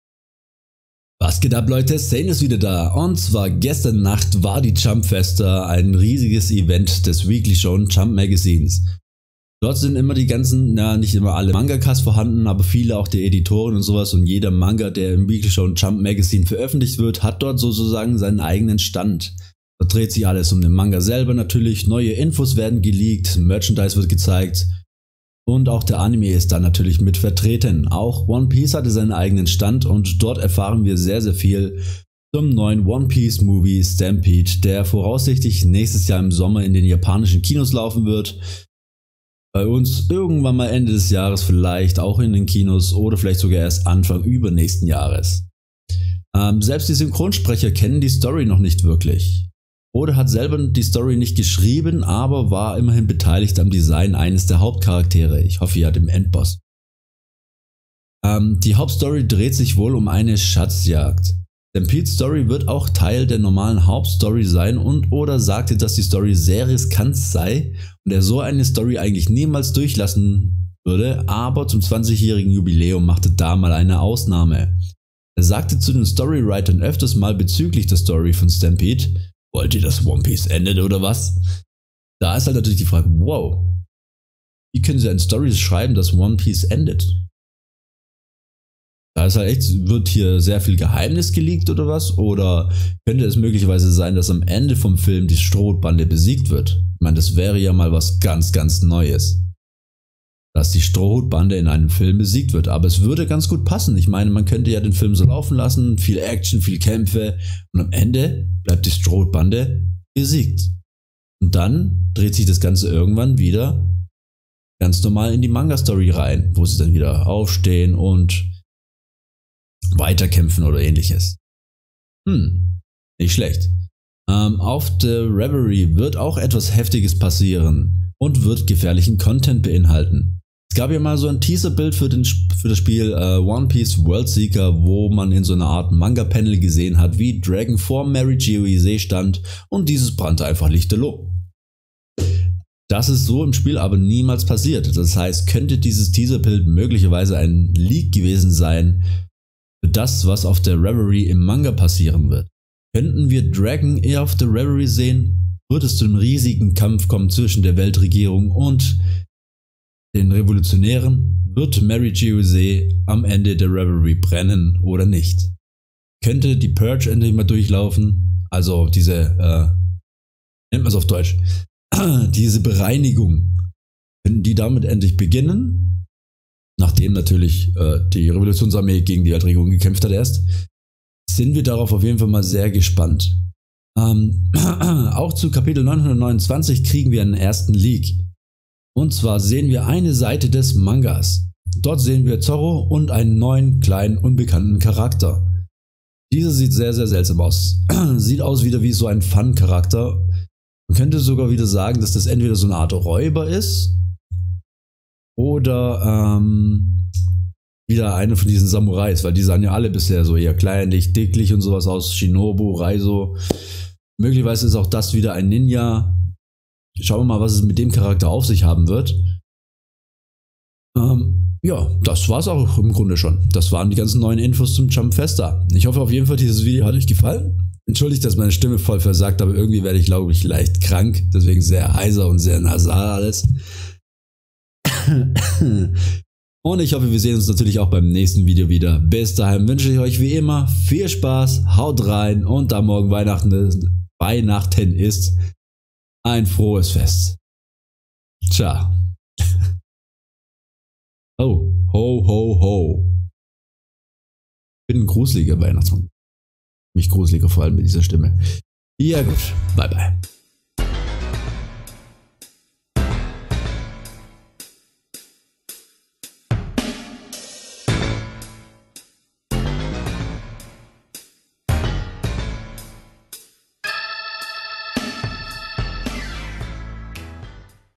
One Piece 929 Spoiler | Neues zu One Piece Stampede | Neues zur Reverie | Jump Festa☠ONE PIECE NEWS
Letzte Nacht war die Jump Festa 2018 des Weekly Shonen Jump Magazines. Dort gab es einen Mini Spoiler zu One Piece Kapitel 929, neue Infos zum One Piece Movie One Piece Stampede und neue Infos zur Reverie!!!

«ONE PIECE»
Eiichiro Oda
Shueisha, 1997

► Animo: FaceFaultTV

MEINE LIVESTREAMS

RECHTLICHES